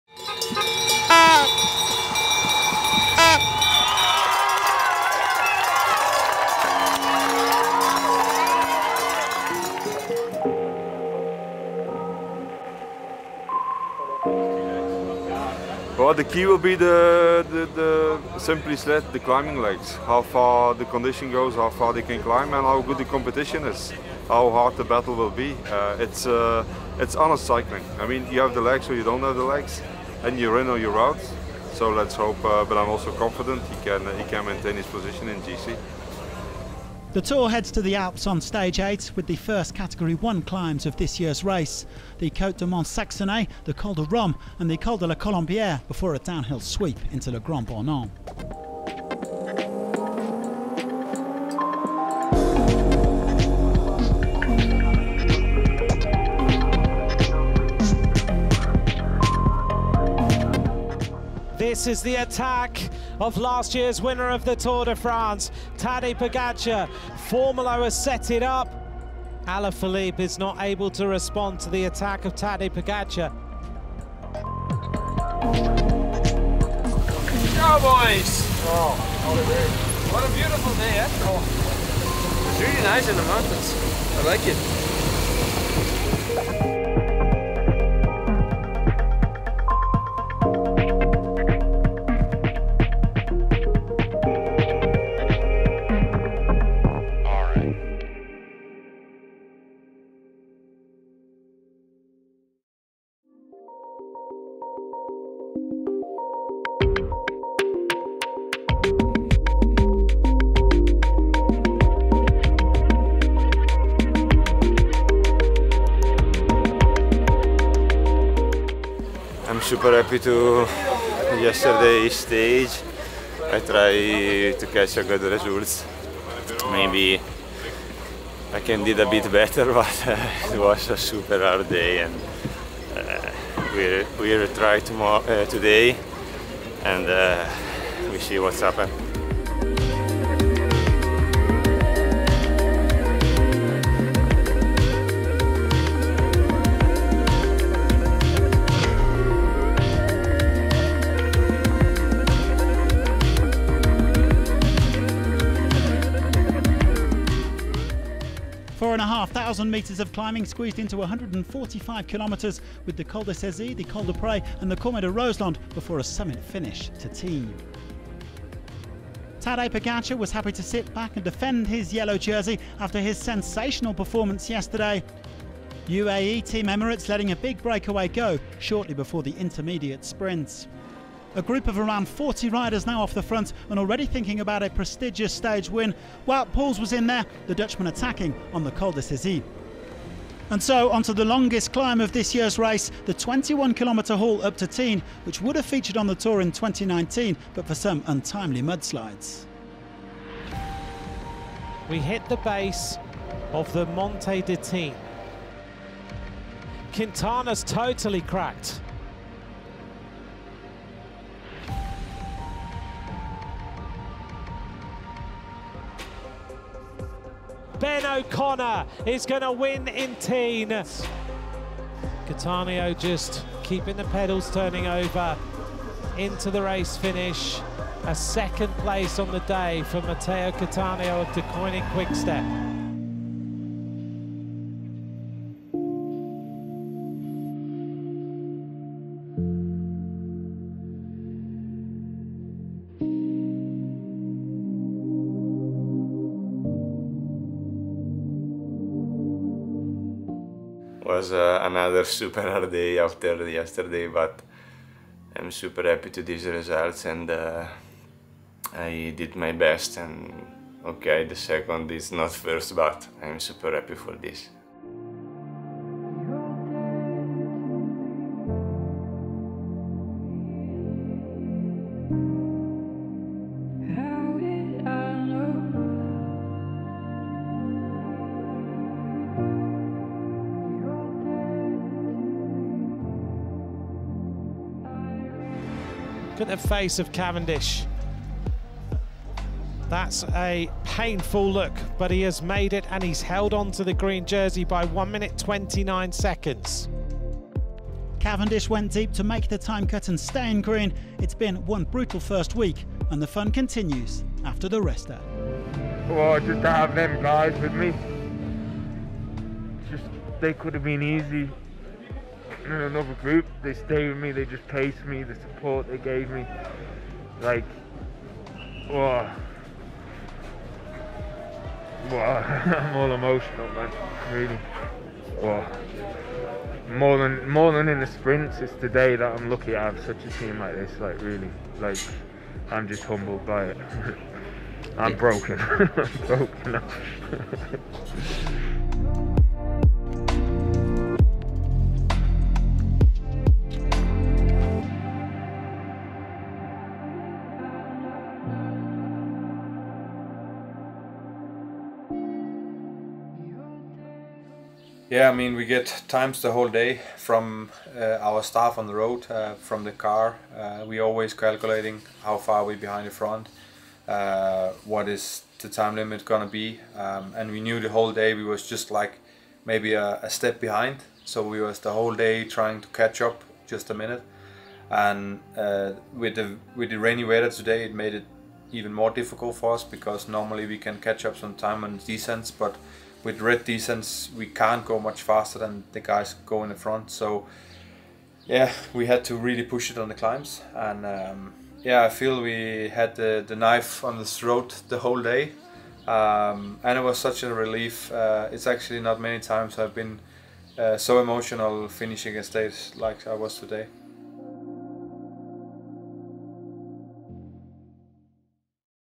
Tour de France: An epic weekend in the mountains
From Mattia Cattaneo’s back-to-back breakaways and maiden podium at the race to Mark Cavendish making the time cut on the draining day to Tignes with the help of an incredible Deceuninck – Quick-Step team, we had an emotionally charged weekend at Le Tour that we won’t forget anytime soon.

Video: Nick Poole